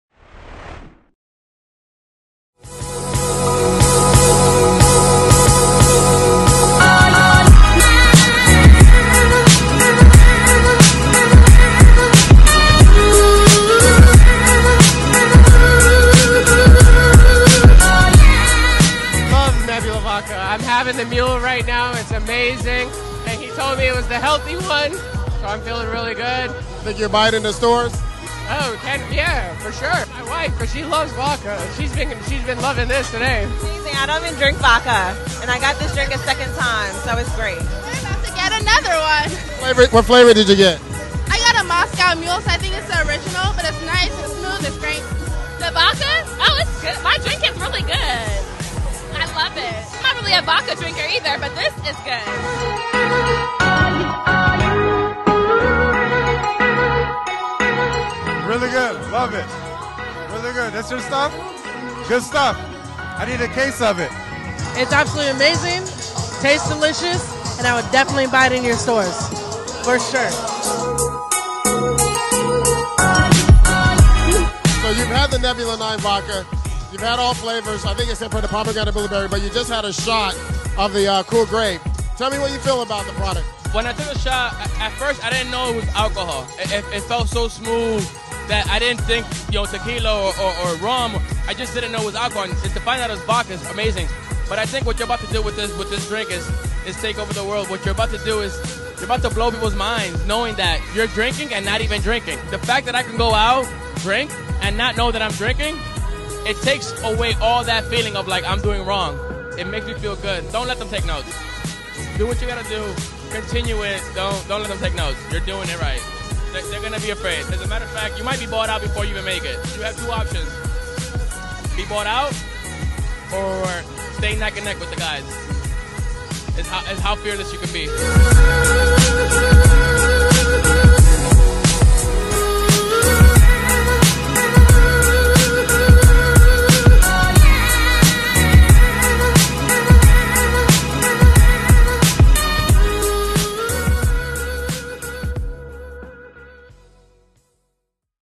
Nebula9Vodka Urban League Derby Party
Neb9 Vodka served some amazing cocktails to some amazing guest for the kickoff of the Kentucky Derby 2018 races.